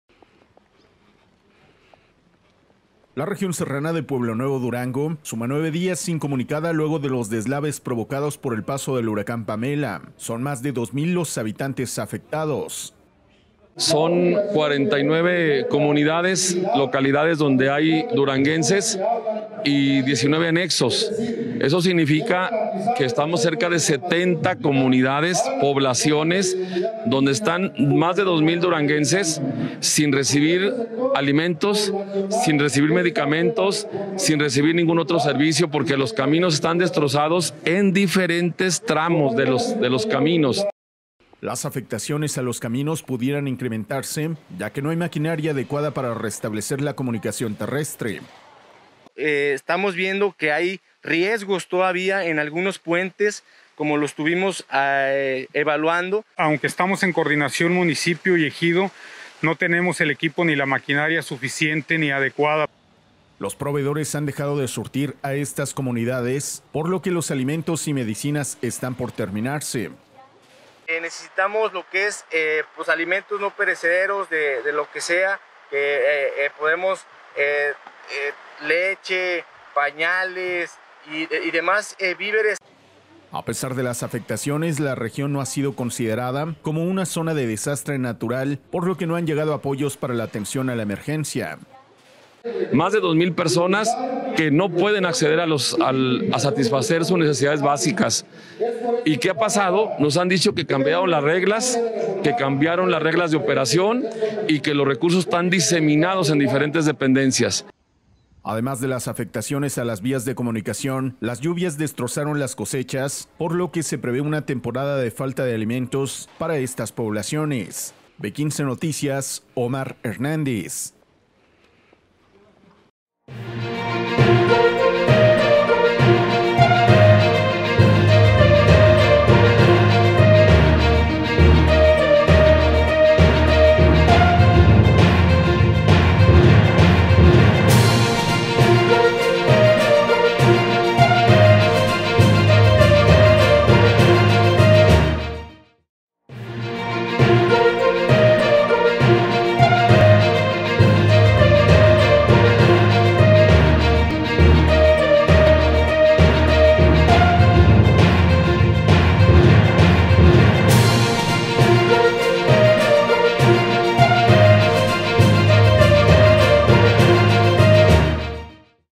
Más de 2 mil duranguenses están incomunicados por destrozos de las lluvias
Pueblo Nuevo, Durango.- La región serrana de Pueblo Nuevo Durango, suma nueve días incomunicados luego de los deslaves provocados por el paso del huracán Pamela… Son más de dos mil los habitantes afectados…

Las afectaciones a los caminos pudieran incrementarse ya que no hay maquinaria adecuada para reestablecer la comunicación terrestre. Los proveedores han dejado de surtir a estas comunidades por lo que los alimentos y medicinas están por terminarse.

A pesar de las afectaciones, la región no ha sido considerada como una zona de desastre natural por lo que no han llegado apoyos para la atención a la emergencia.

Además de las afectaciones a las vías de comunicación las lluvias destrozaron las cosechas por lo que se prevé una temporada de falta de alimentos para estas poblaciones.